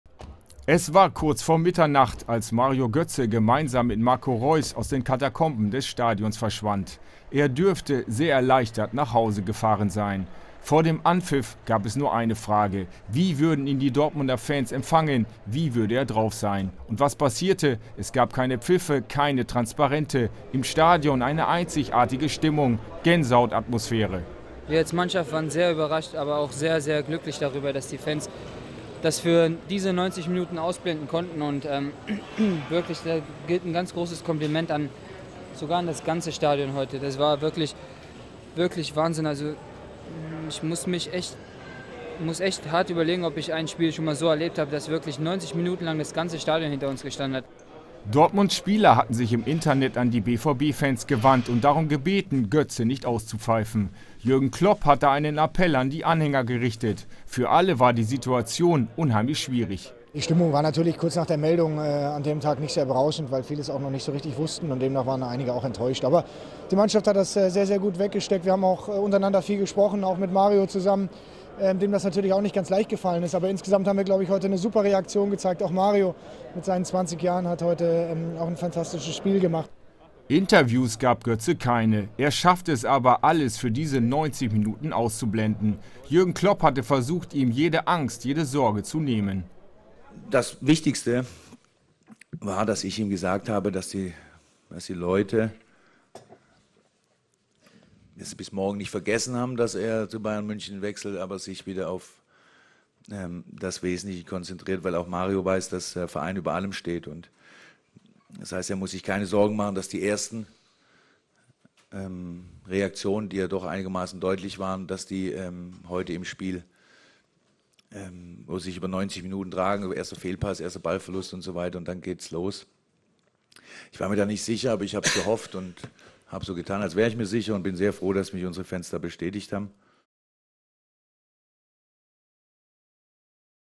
Götze: Erleichterung an allen Fronten .. . kicker.tv
Vor dem Anpfiff knisterte das ganze Stadion. Wie würden die BVB-Fans den zum FC Bayern abwandernden Mario Götze empfangen? Der Mittelfeldspieler wurde mit warmem Applaus empfangen, leitete in der achten Minute das 1:0 ein. Alle Dortmunder Profis lobten das Verhalten der Zuschauer auf den Rängen, Marcel Schmelzer hatte eine solche umfassende Unterstützung noch nie erlebt.

K1268313
Dt.Fußball, 1.Bundesliga, Champions League, Borussia Dortmund, Real Madrid, Marcel Schmelzer, Jürgen Klopp, Sebastian Kehl